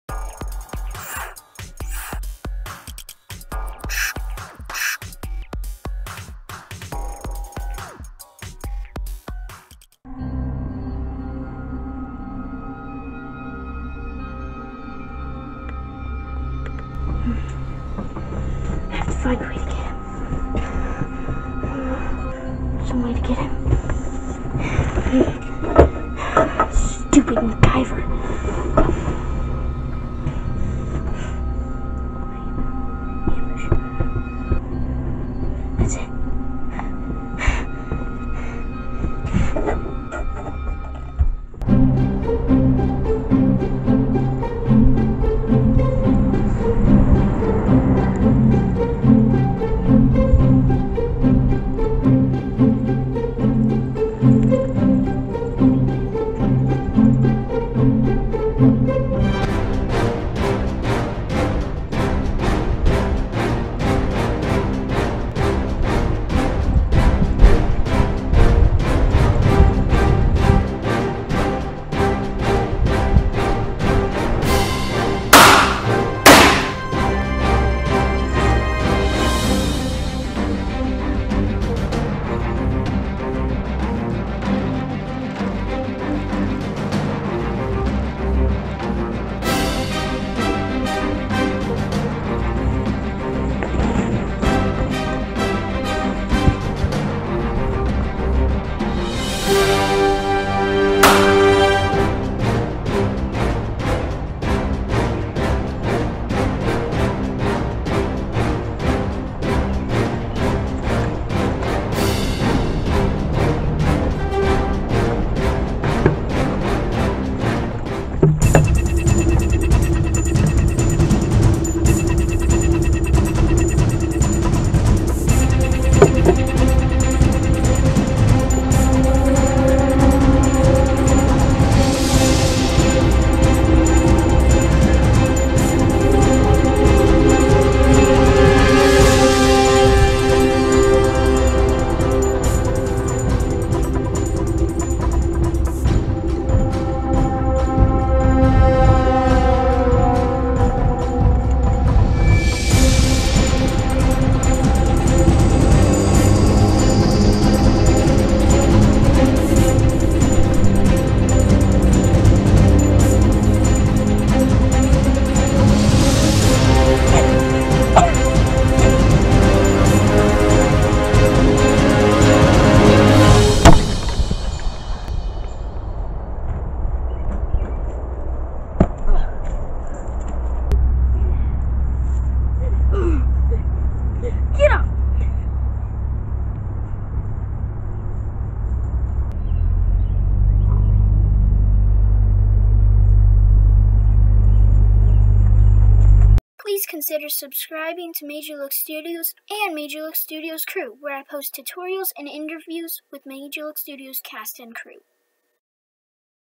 MacGyver: Hunted
In the classic style of the original Made Ya Look Series, MacGyver is hunted by an old enemy seeking revenge. Cast and Crew: Cody and Austin.